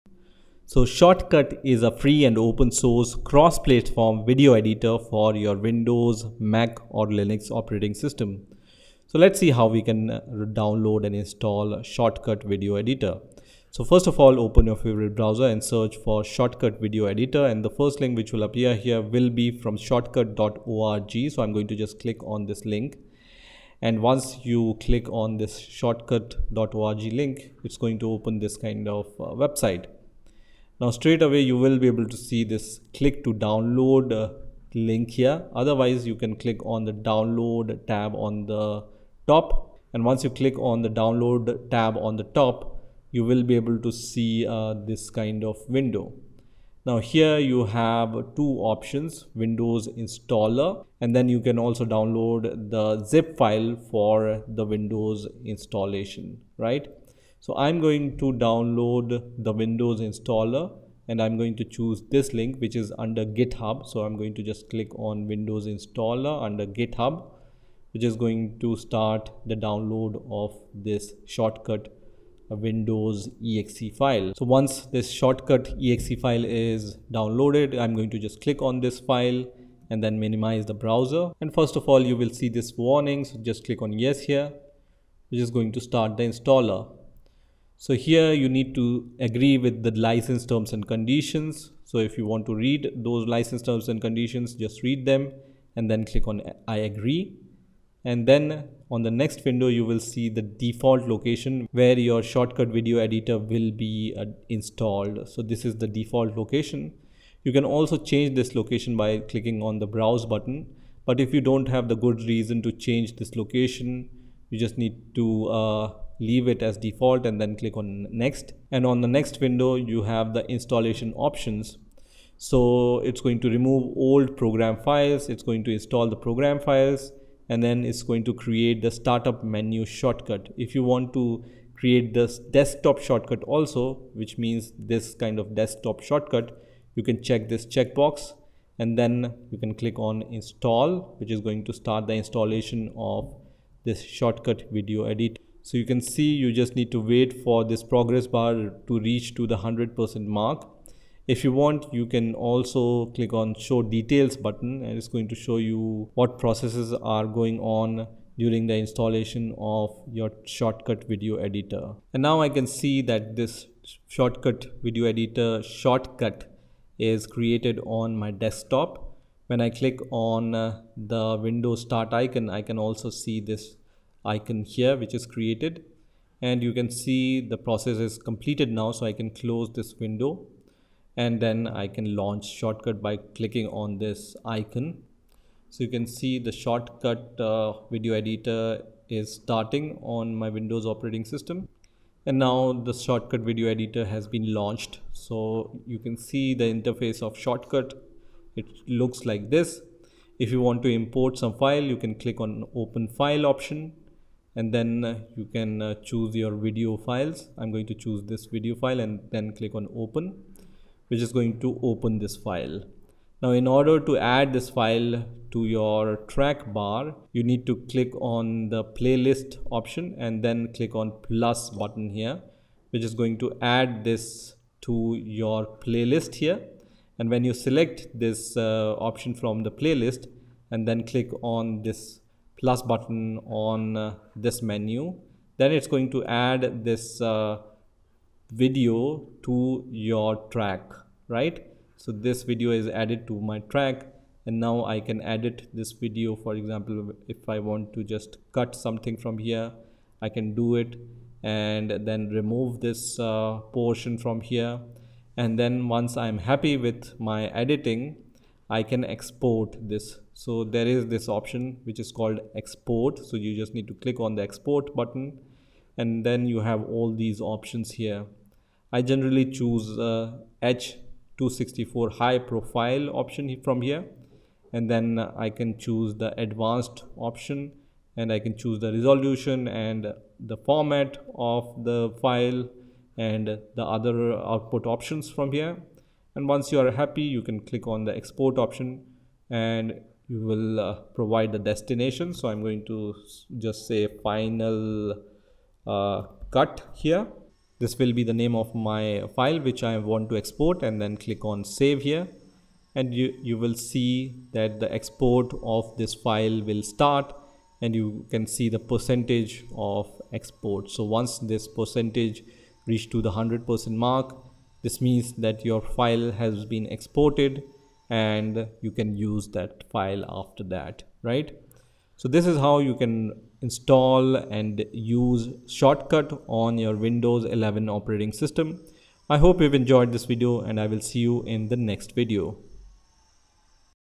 How to install FREE Video Editor on Windows 11
In this video we are going to see How to Download and Install Shotcut Video Editor on Windows 11 PC or Laptop. Shotcut Video Editor is a free and  open source, cross-platform editor for Windows, Mac and Linux.

topics addressed in this tutorial:
shotcut video editor tutorial
free video editing software
openshot video editor download
shotcut video editor 32-bit 64-bit
shortcut video downloader